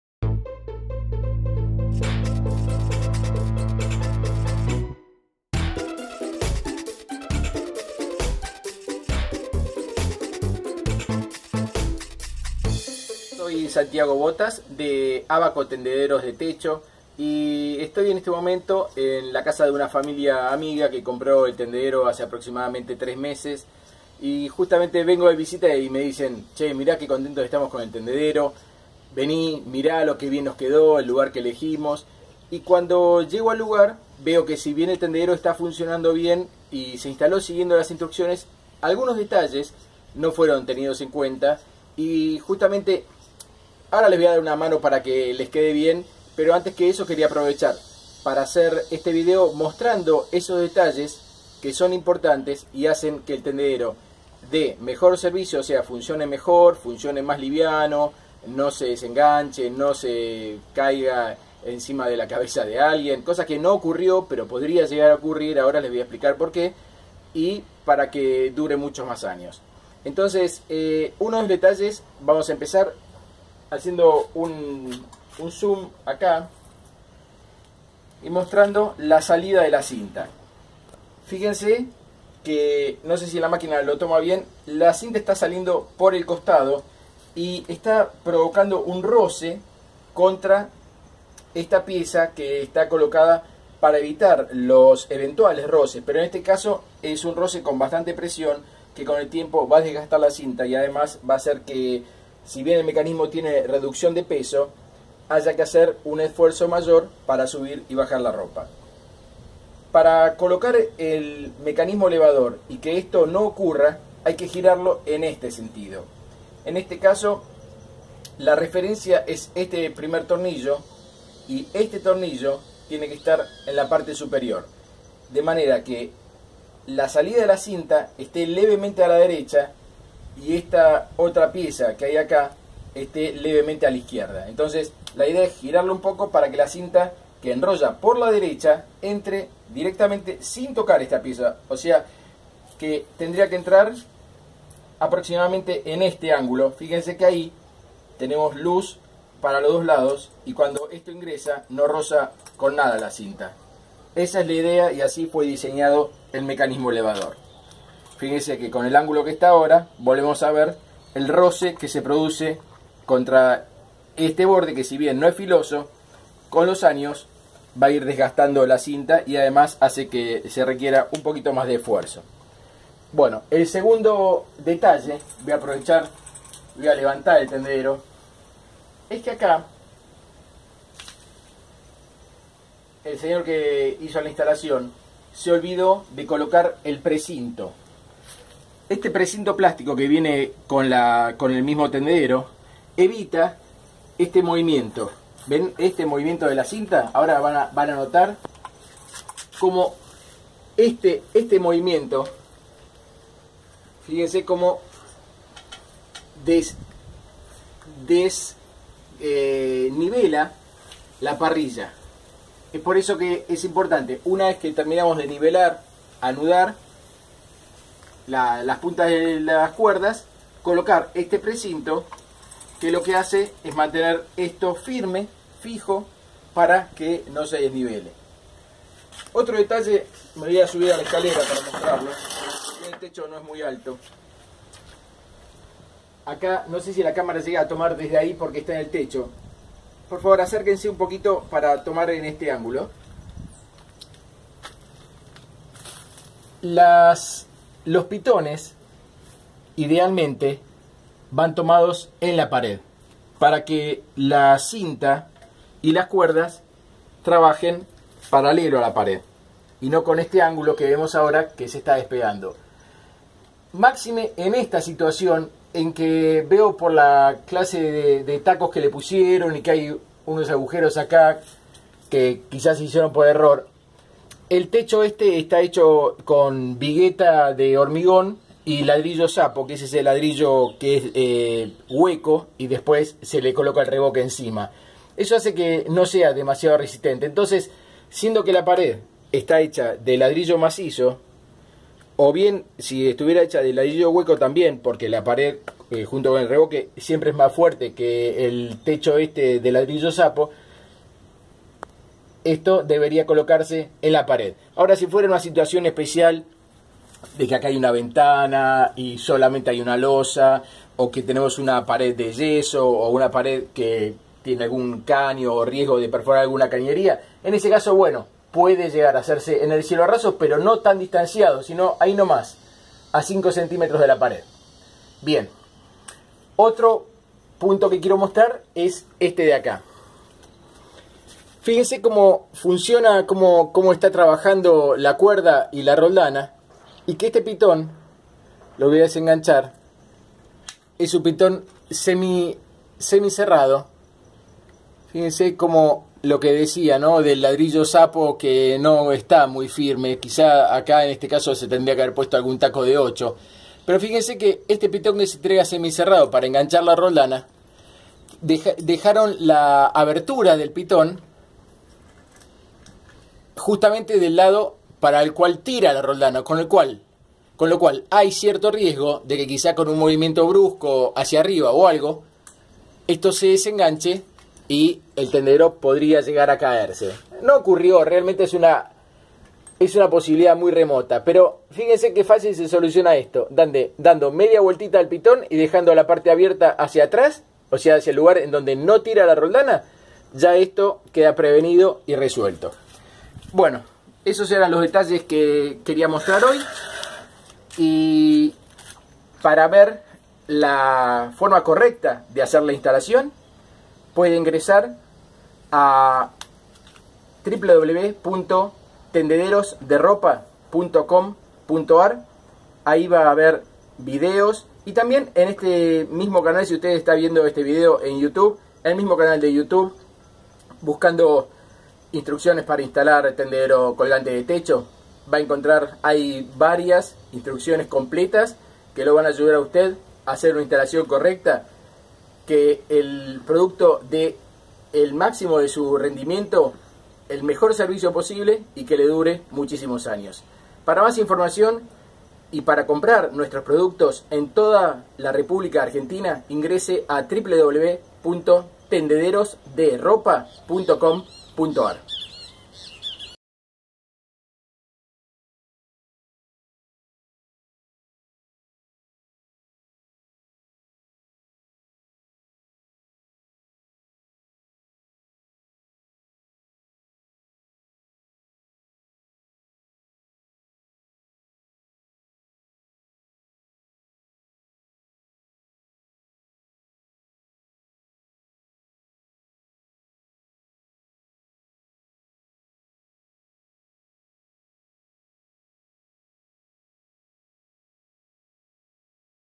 Errores a evitar en la instalación de un tender de techo
Si el tendedero colgante no funciona correctamente revise los detalles que se muestran en el video.
Errores a evitar cuando se realiza la instalación de un tendedero colgante de techo